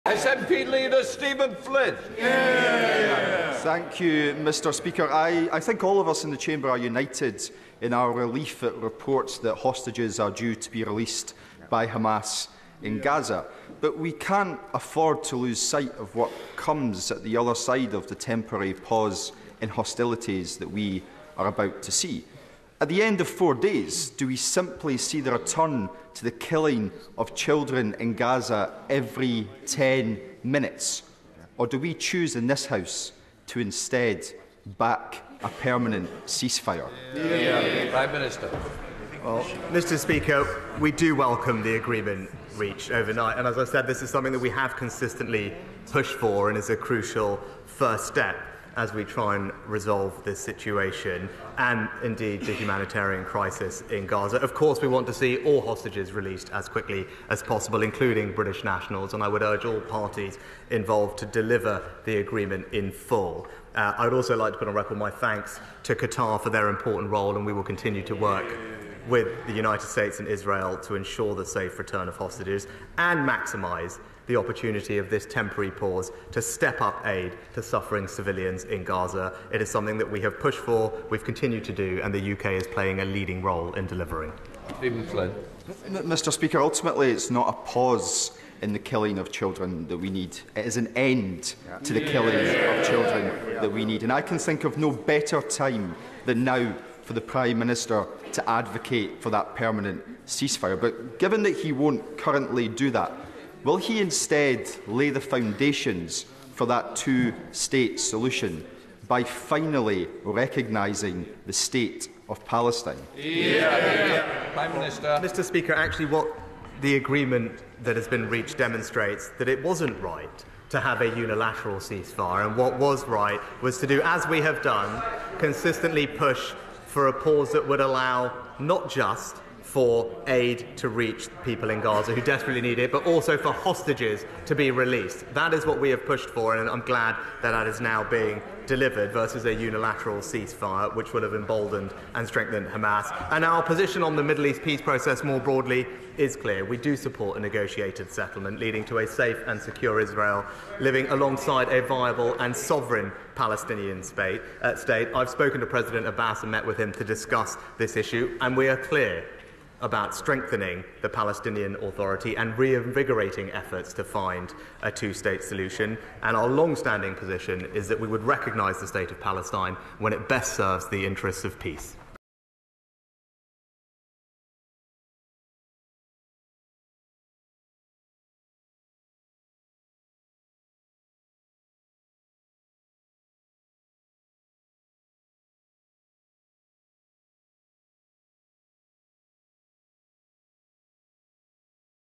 SNP Leader calls on Sunak to recognise Palestinian state at PMQs
Stephen Flynn continues to push Rishi Sunak to support an end to the violence in Gaza.